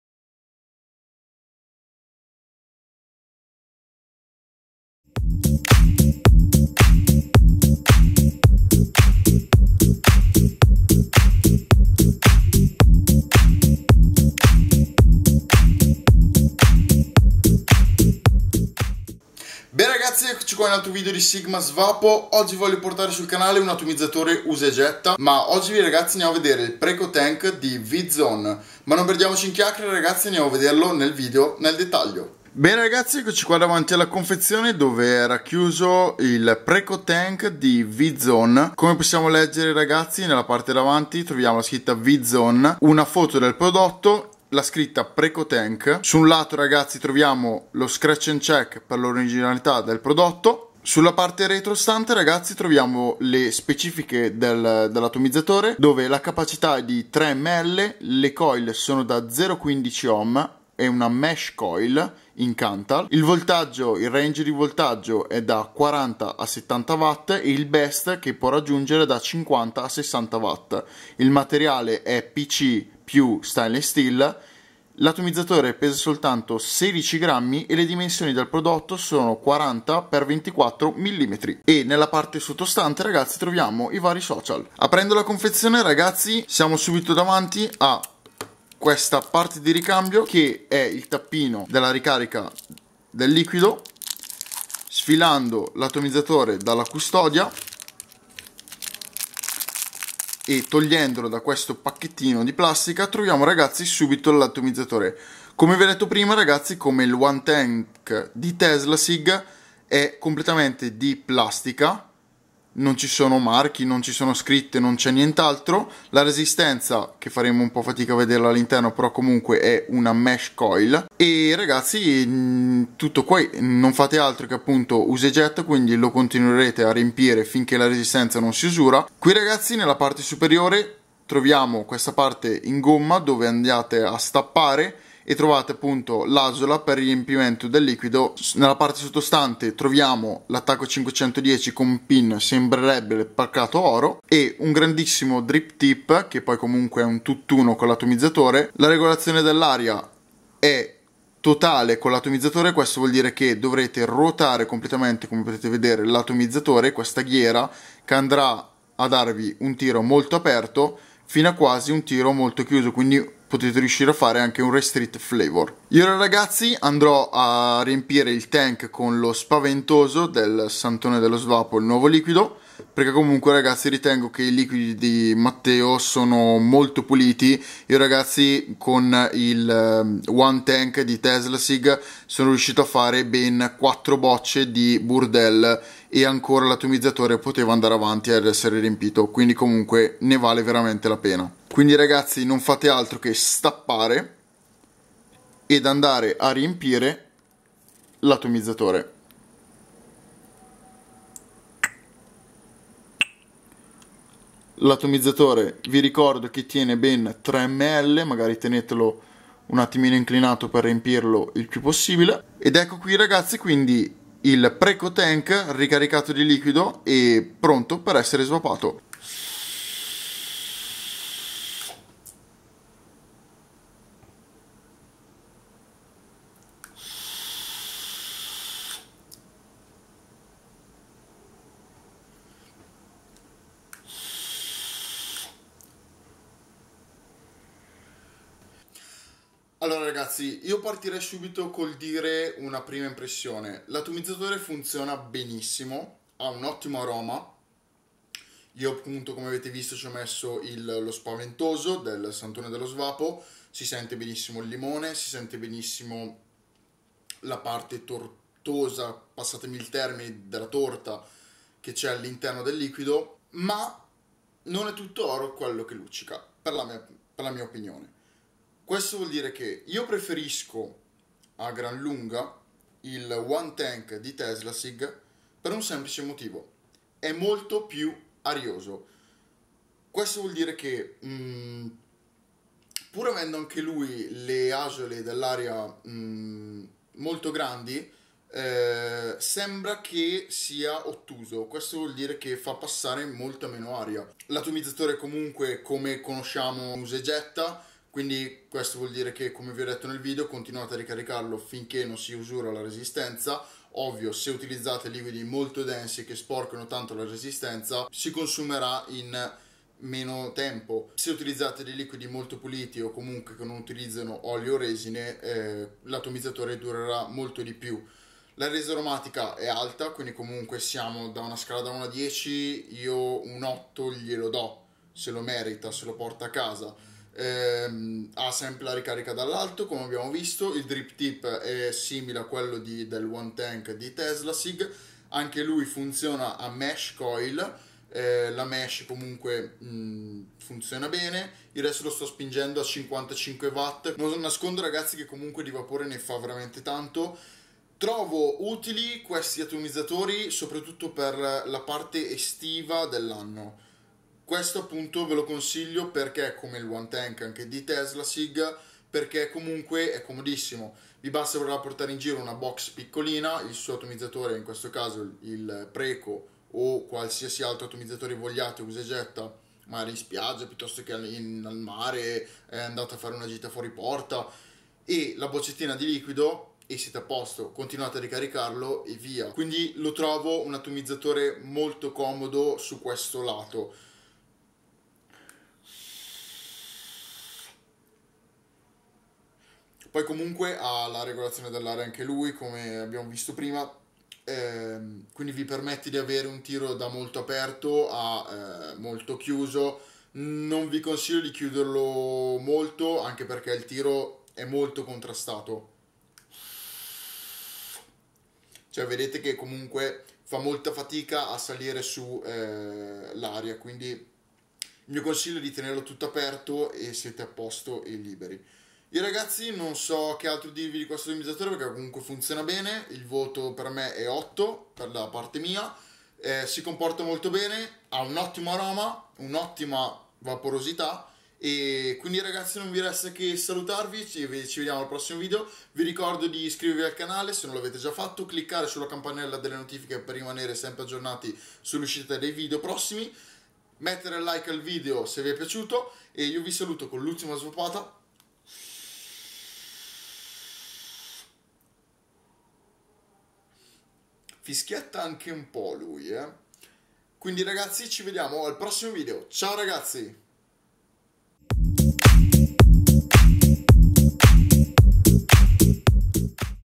PRECO TANK BY V ZONE
DESCRIZIONE PRODOTTO :

Già riconosciuto sul mercato del vaping per le sue originalissime box, il produttore cinese Vzone lancia oggi un nuovo atomizzatore sul mercato: il Preco Tank.

Più che un semplice atom, il tank Preco è un vero concetto o addirittura una rivoluzione!
Interamente progettato con plastica alimentare, non si apre, non può essere smontato e sarà utilizzato come un qualunque ricaricabile. Trasparente, con un design semplice ma sobrio, il Preco non troverà spazio per per i neofiti dello svapo, sia per i vapers più esperti.